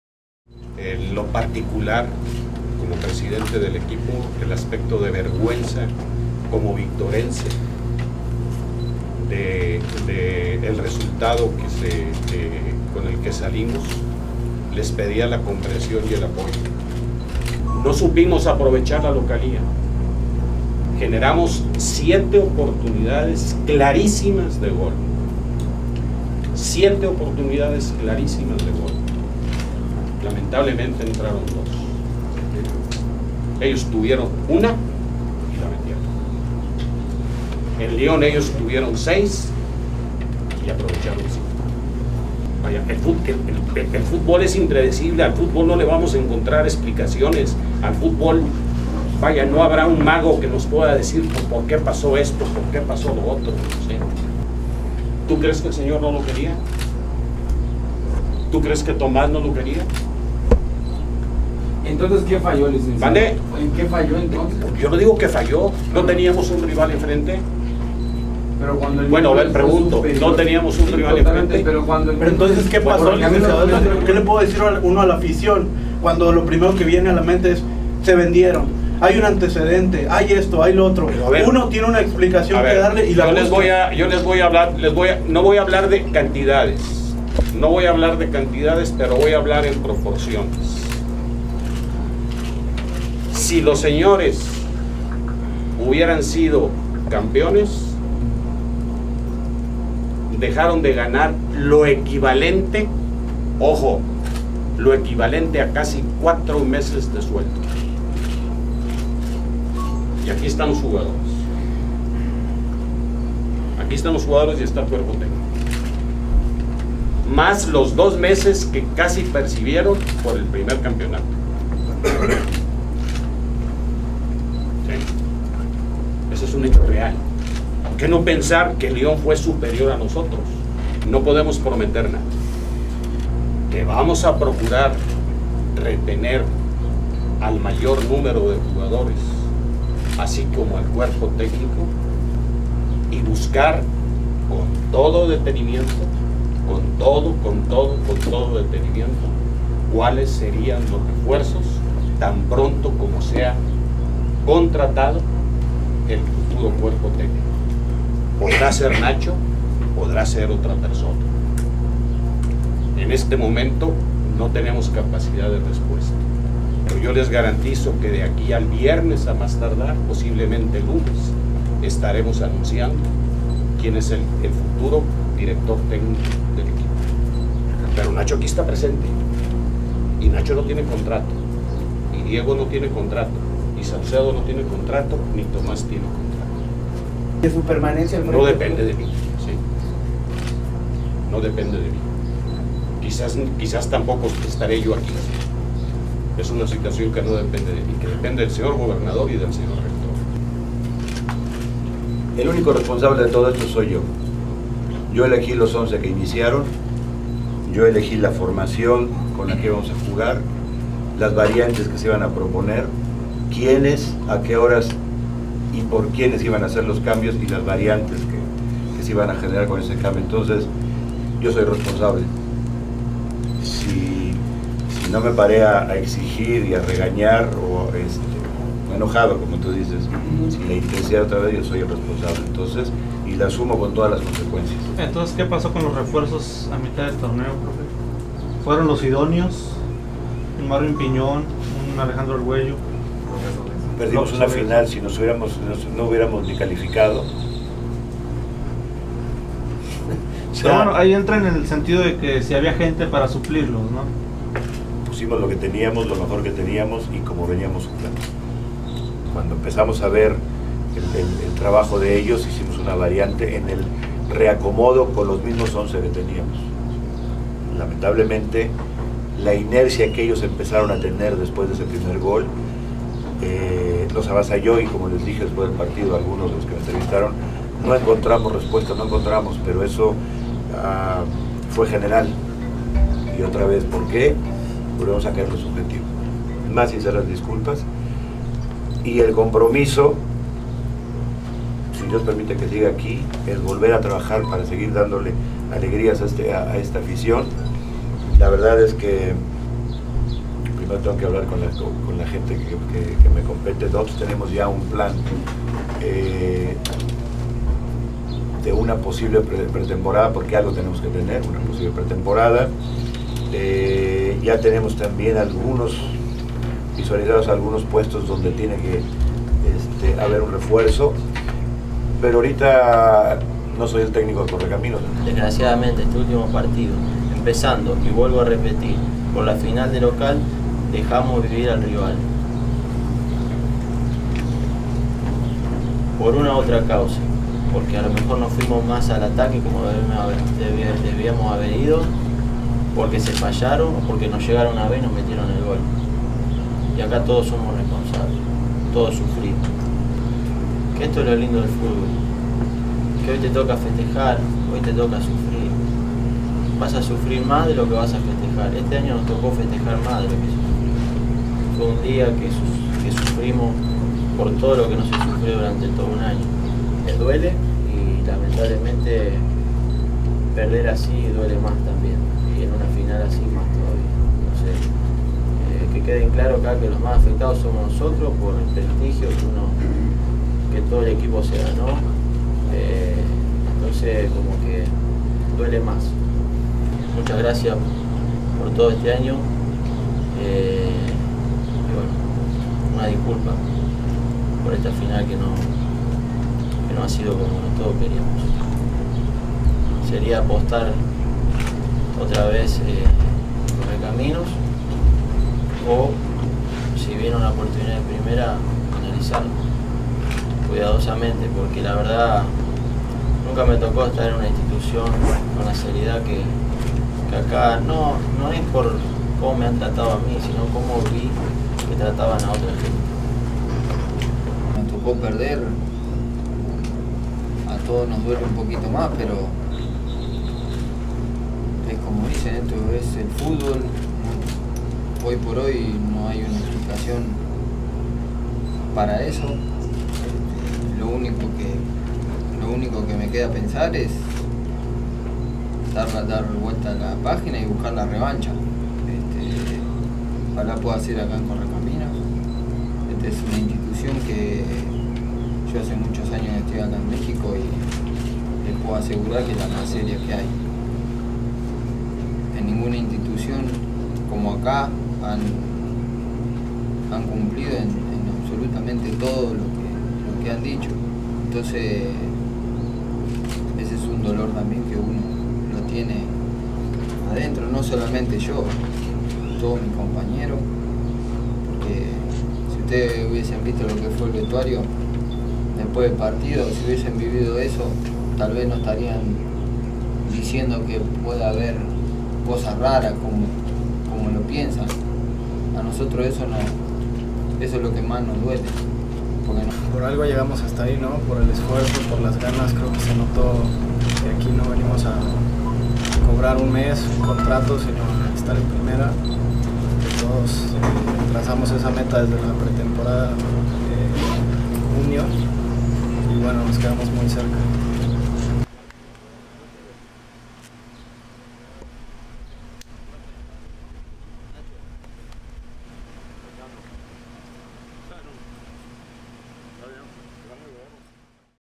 ¿Qué le pasó a Correcaminos?
En conferencia de prensa, eI presidente del club de fútbol Correcaminos, Francisco Filizola, el entrenador "Nacho" Rodríguez junto a los jugadores Diego Olsina, Roberto Nicolàs Saucedo y Tomás Domínguez expresaron su sentir respecto al objetivo truncado de ascender a primera división nacional.